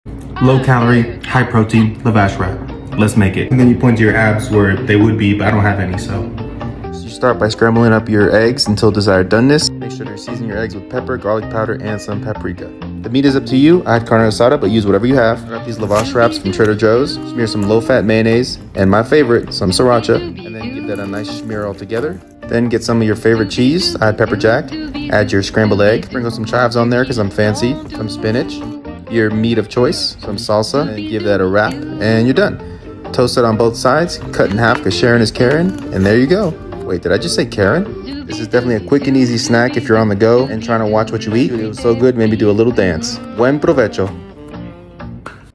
High Protien Lavash Wraps!
A healthy-ish meal that’s suitable for breakfast lunch or dinner! Recipe is super easy and flexible, let me know if you make it!

Buen provecho!!